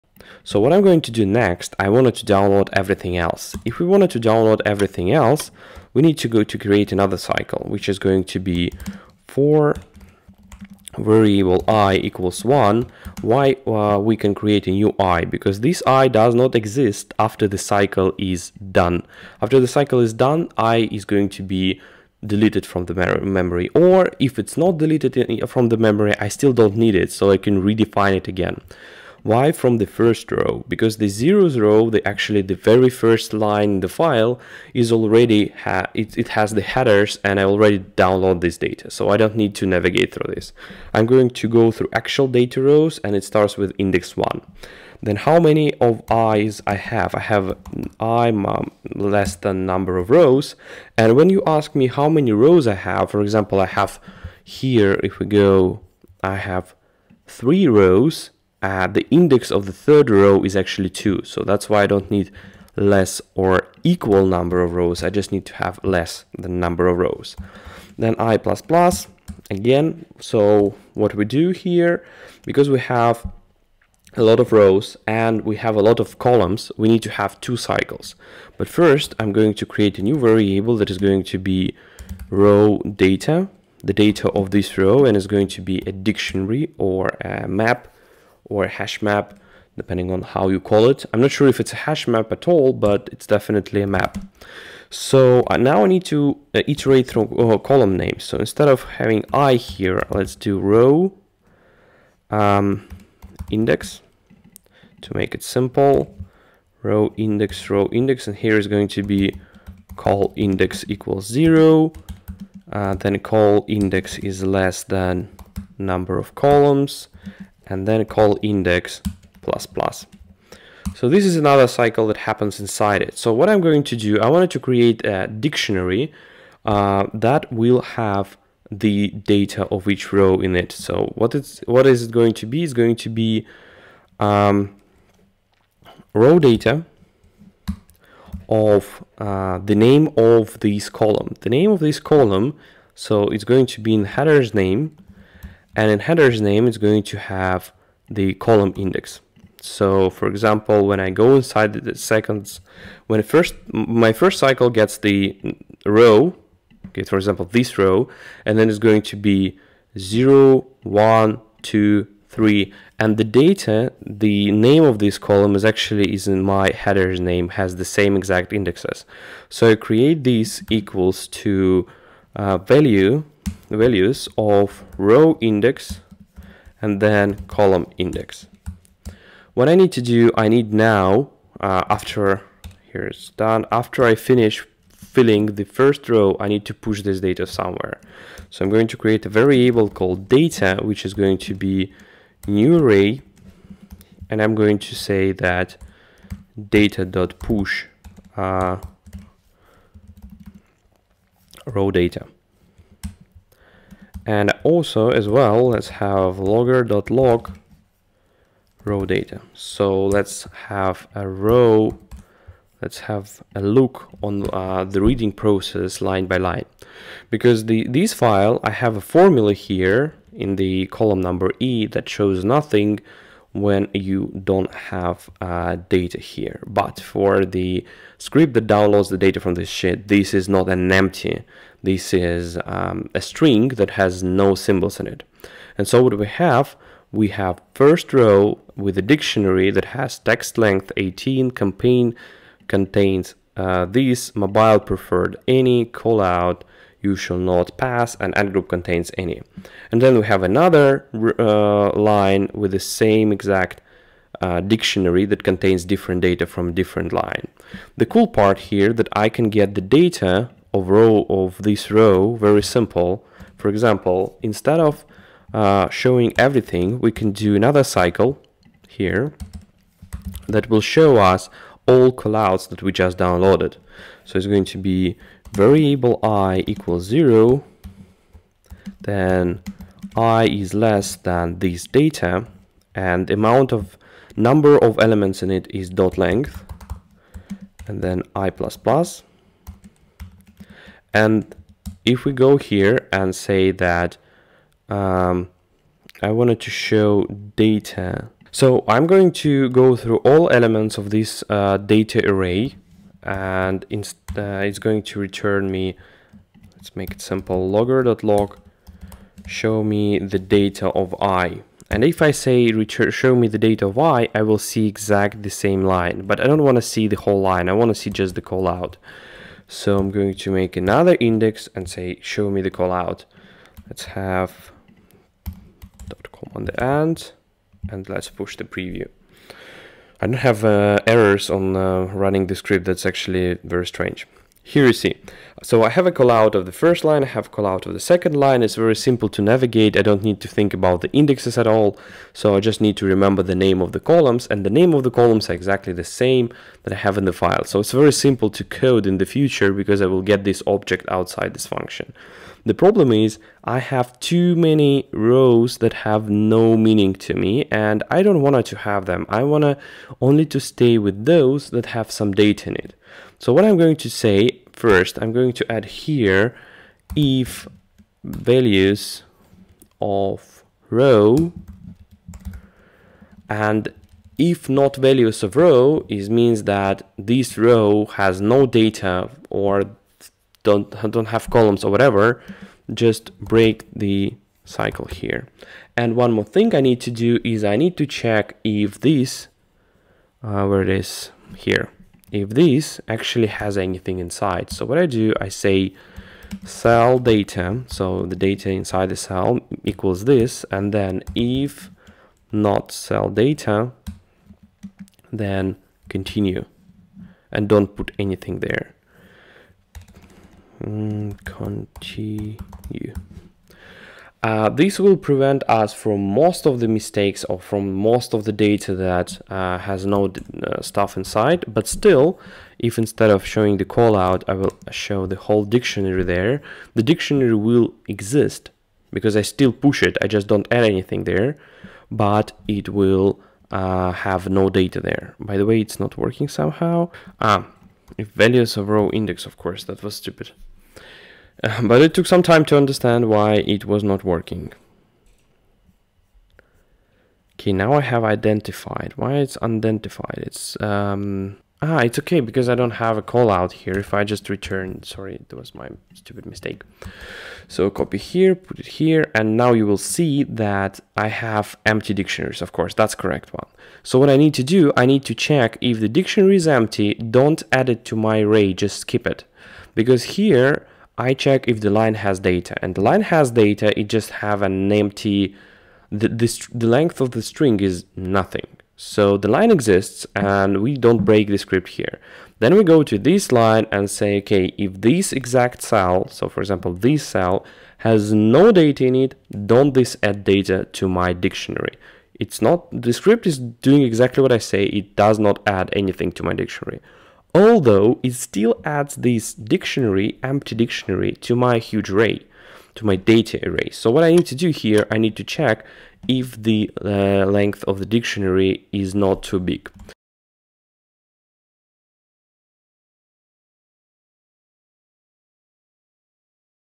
Google Ads Scripts - Loading Data from Google Sheets (GAS #10)
First part of building a Callout is to download data from Google Sheets file.

Have questions? Ask them in comments!